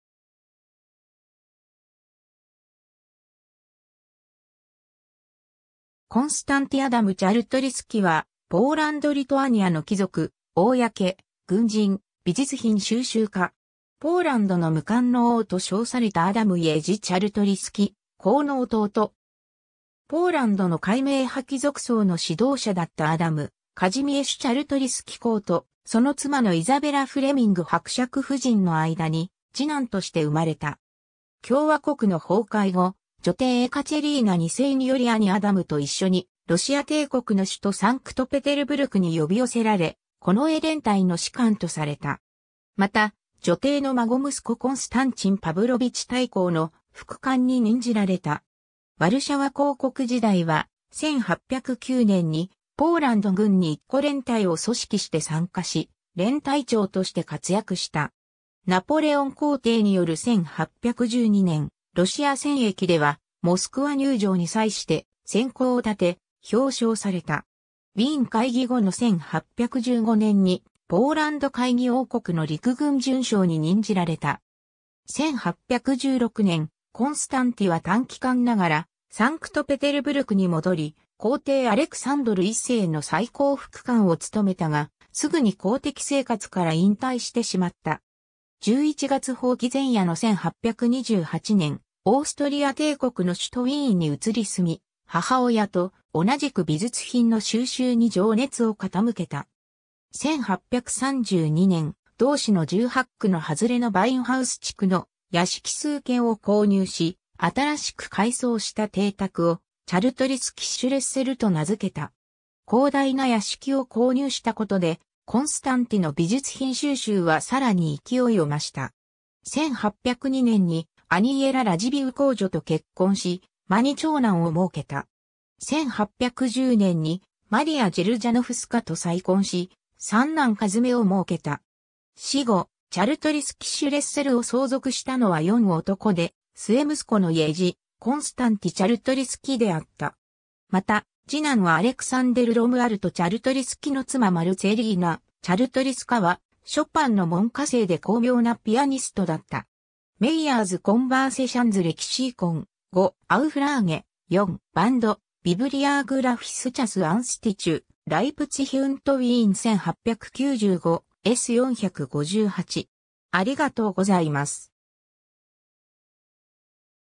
コンスタンティ・アダム・チャルトリスキ
アダム・カジミェシュ・チャルトリスキの子女
コンスタンティ・アダム・チャルトリスキ（Konstanty Adam Czartoryski, 1774年10月28日 - 1860年4月23日）は、ポーランド・リトアニアの貴族、公（Książę）、軍人、美術品収集家。
「ポーランドの無冠の王」と称されたアダム・イェジ・チャルトリスキ公の弟。
 ポーランドの開明派貴族層の指導者だったアダム・カジミェシュ・チャルトリスキ公と、その妻のイザベラ・フレミング伯爵夫人の間に次男として生まれた。
共和国の崩壊後、女帝エカチェリーナ2世により兄アダムと一緒にロシア帝国の首都サンクトペテルブルクに呼び寄せられ、近衛連隊の士官とされた。
また女帝の孫息子コンスタンチン・パヴロヴィチ大公の副官に任じられた。
ワルシャワ公国時代は、1809年にポーランド軍に1個連隊を組織して参加し、連隊長（大佐）として活躍した。
ナポレオン皇帝による1812年ロシア戦役では、モスクワ入城に際して戦功を立て、表彰された。
 ウィーン会議後の1815年にポーランド会議王国の陸軍准将に任じられた。
1816年、コンスタンティは短期間ながらサンクトペテルブルクに戻り、皇帝アレクサンドル1世の最高副官を務めたが、すぐに公的生活から引退してしまった。
11月蜂起前夜の1828年、オーストリア帝国の首都ウィーンに移り住み、母親と同じく美術品の収集に情熱を傾けた。
1832年（ないし1834年）、同市の18区（ヴェーリンク区）の外れのヴァインハウス地区の屋敷数軒を購入し、新しく改装した邸宅をチャルトリスキ・シュレッセルと名付けた。
広大な屋敷を購入したことで、コンスタンティの美術品収集はさらに勢いを増した。
 1802年にアニェラ・ラジヴィウ公女（1781年 - 1808年）と結婚し、間に長男をもうけた。
1810年にマリア・ジェルジャノフスカ（1790年 - 1842年）と再婚し、三男一女をもうけた。
死後、チャルトリスキ・シュレッセルを相続したのは四男で末息子のイェジ・コンスタンティ・チャルトリスキ（1828年 - 1912年）であった。
また、次男アレクサンデル・ロムアルト・チャルトリスキ（1811年 - 1886年）の妻マルツェリーナ・チャルトリスカは、ショパンの門下生で高名なピアニストだった。
 ^ Meyers Konversations-Lexikon, 5. Auflage, 4. Band, Bibliographisches Institut, Leipzig und Wien 1895, S. 458